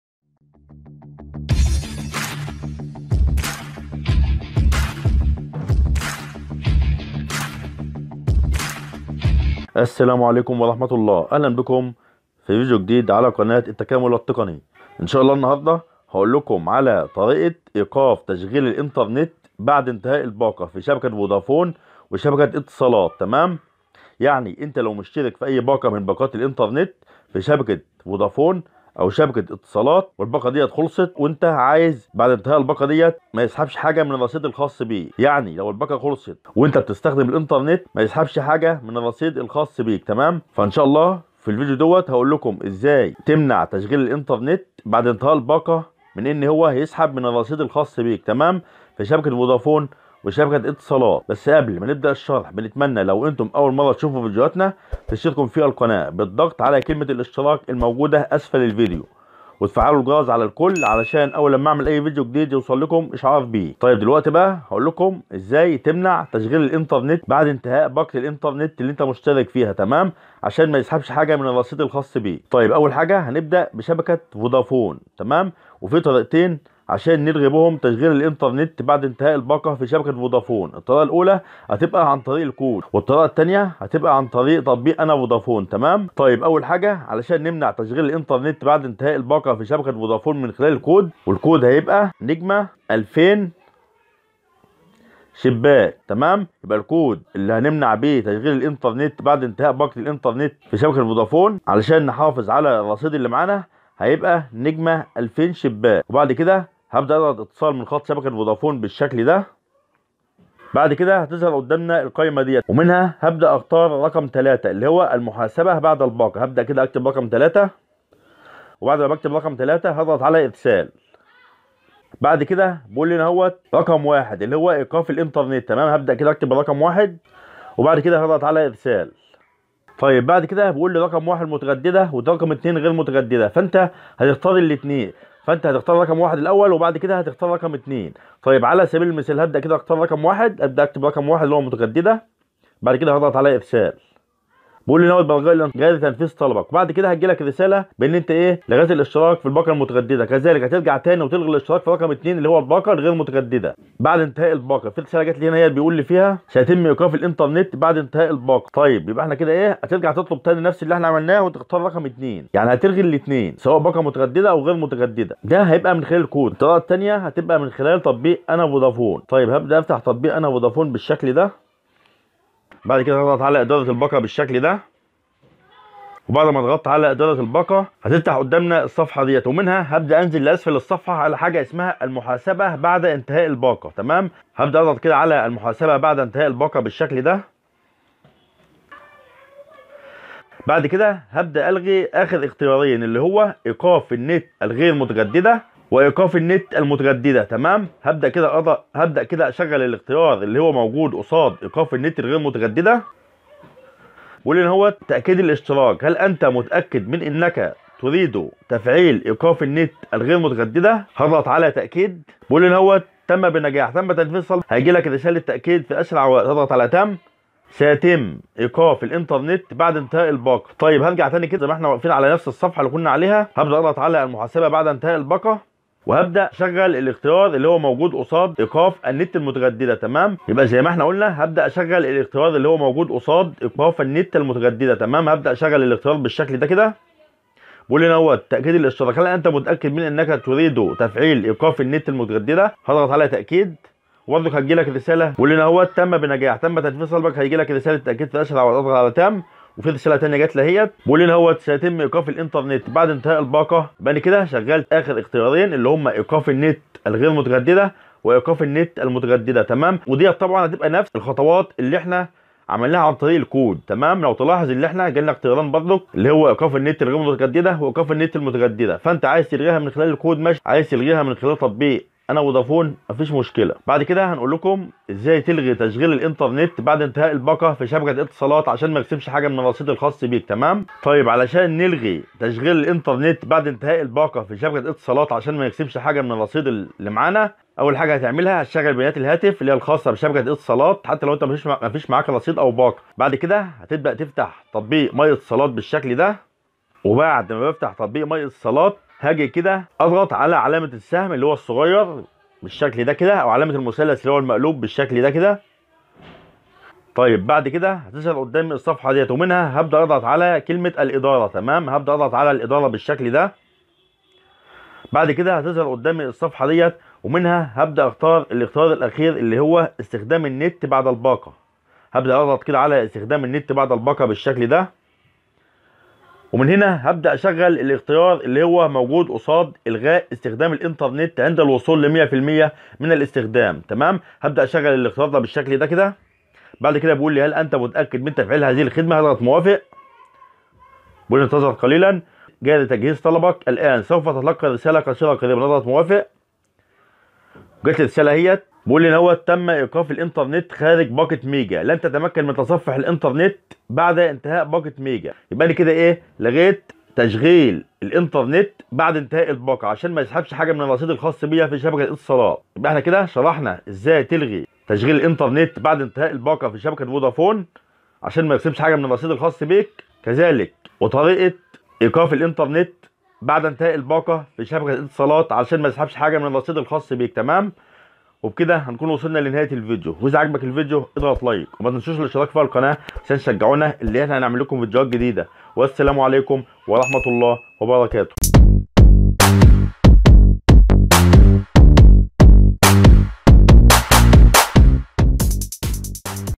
طريقة ايقاف تشغيل الانترنت بعد انتهاء الباقة فودافون واتصالات لعدم نفاذ الرصيد
طريقة ايقاف الانترنت بعد انتهاء الباقة لكى لا يسحب من الرصيد فى شبكة فودافون وشبكة اتصالات 2022
كود ايقاف تشغيل الانترنت بعد انتهاء الباقة فى شبكة اتصالات للحفاظ على الرصيد.
طريقة ايقاف خدمة الانترنت بعد انتهاء الباقة فى شبكة فودافون للحفاظ على الرصيد.